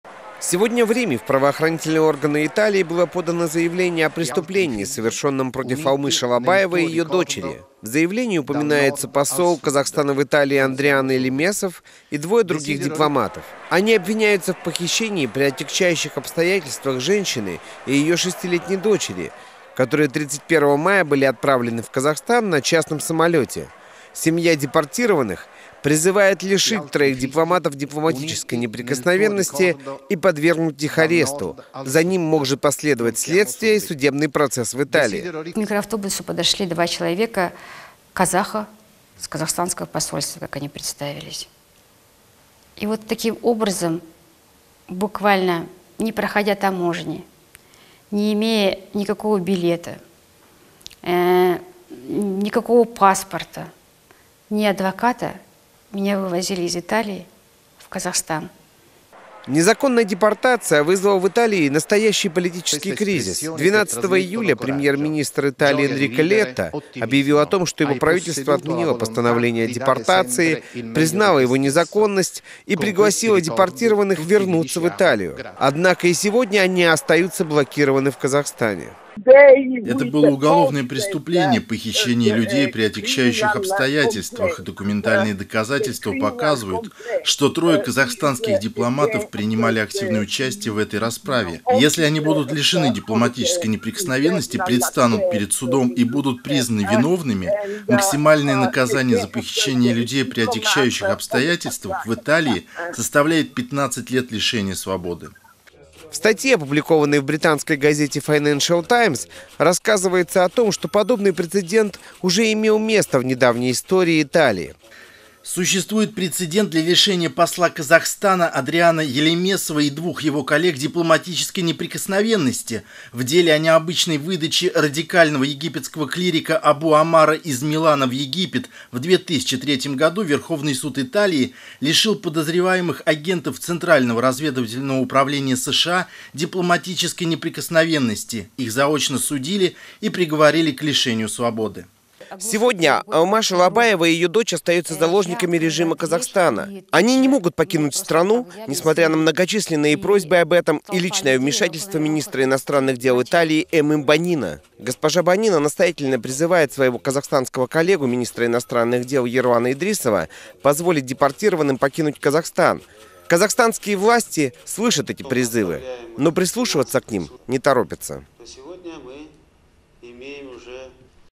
Преследования дипломатов Казахстана в Италии / 1612
Итальянские адвокаты подали заявление о преступлении казахстанских дипломатов в Италии.